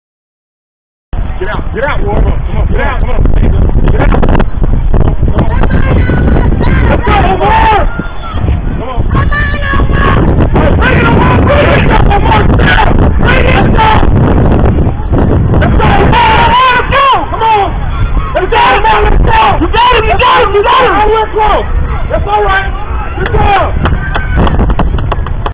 Omar Craddock 200m dash (21.78 sec)
Running the 200 in Temple for a taaf meet. Came in second.